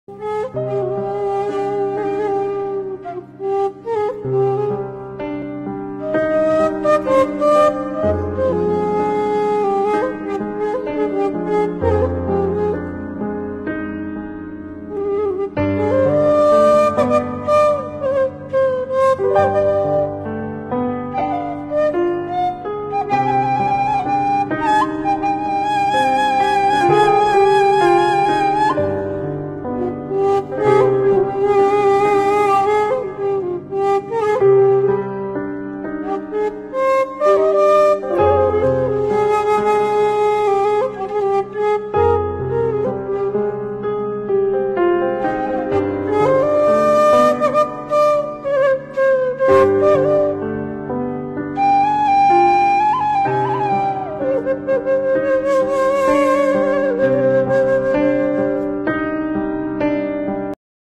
HARMONI KEINDAHAN SULING SUNDA & ALAM PARAHYANGAN
GELIKAN SULING MERE PEPELING

GUNUNG ULAH DILEBUR
LEBAK ULAH DI RUSAK
URANG JAGA
URANG RIKSA
GEUS JADI TUGAS URANG SALEREA

ENDAHNA ALAM PASUNDAN
ULAH LEUNGIT KU WANCI
ULAH ILANG KU MANGSA

ALLAH ANU ASIH
ALAM  NU NGASAH
MANUSA ANU NGASUH

SALAM.